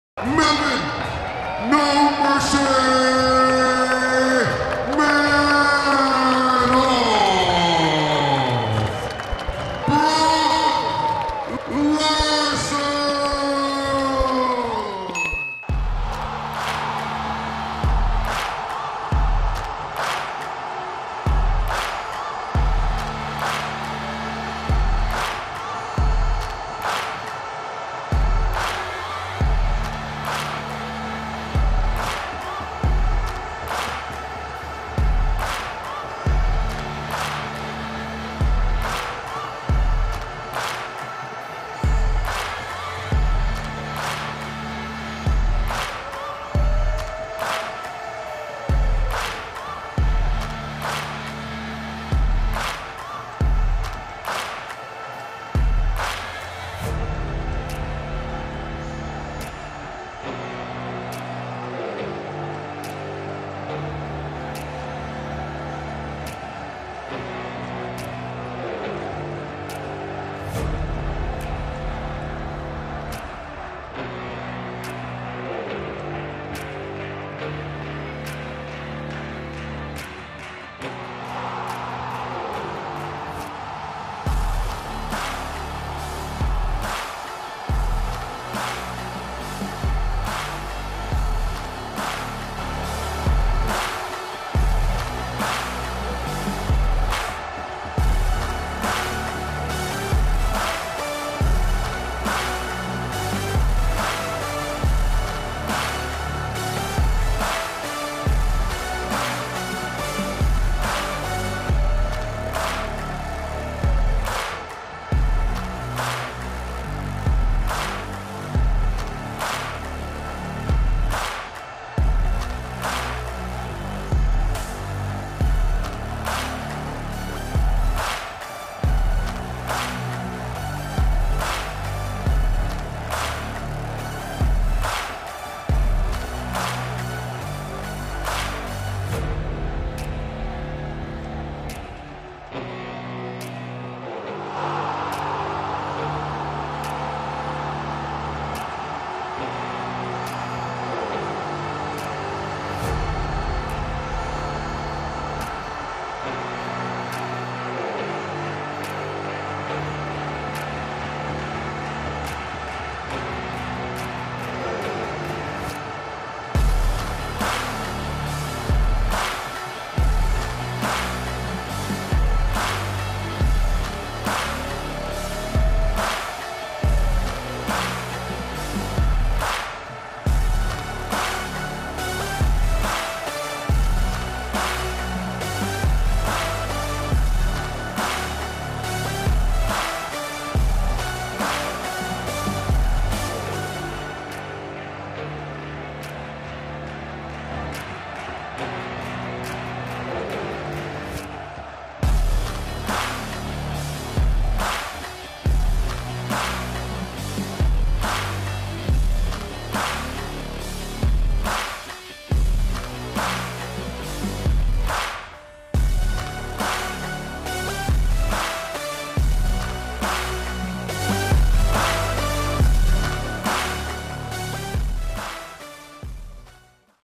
لماذا هرب؟! melvin manhoef vs brock larson نزال مجنون 😠
نزال مجنون بين مقاتلين في الوزن المتوسط مانهوف و لارسون عندما هرب لارسون من مانهوف في الهلب ليتحول الهروب الى فوز في النهاية تابع الملخص حتى النهاية